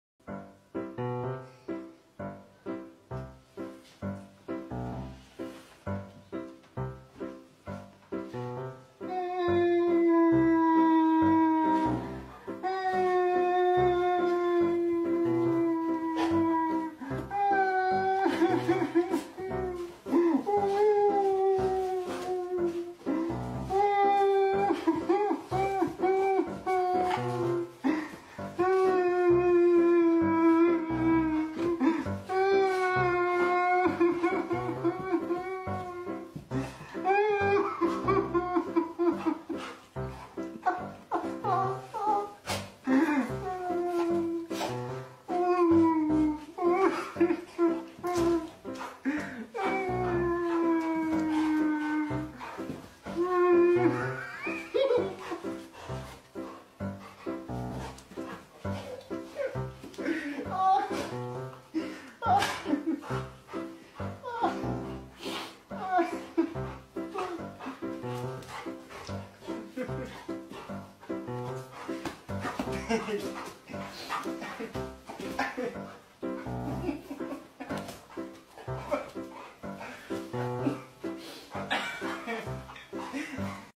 Sahibi ağlayınca üzülen köpek
inanılmaz köpek